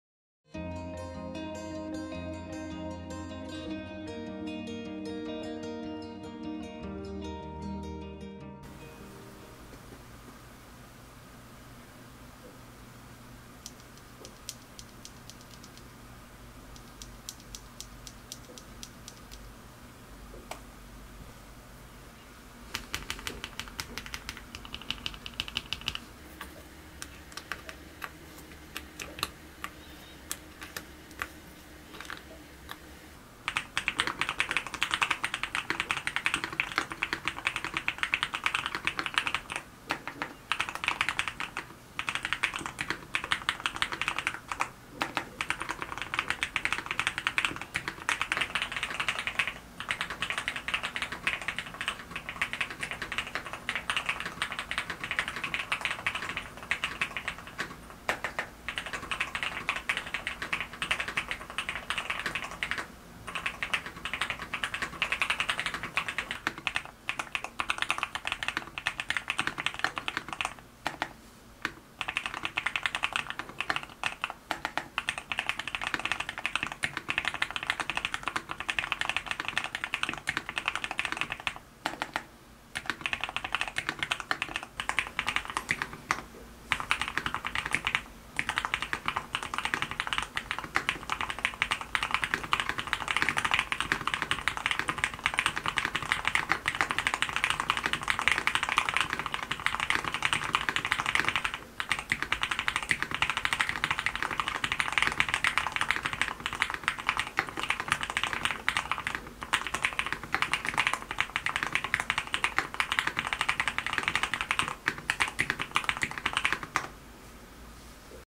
Geon F1-8X 722 RED + Sea Salt switch 상컬트 722와 바다소금 스위치 타건 소리
가성비 스위치로 엄청 유명한 바다소금 스위치를 윤활 후 F1-8X 722 RED와 조합하여 타건해봤습니다.
100알에 2만원 정도라는 게 믿기지 않을 만큼 가성비가 훌륭합니다.
도각거리는 소리도 좋고 약간의 꾸덕함도 있습니다. 노스폴라 스위치 대비 꾸덕함이 약간 약한 느낌?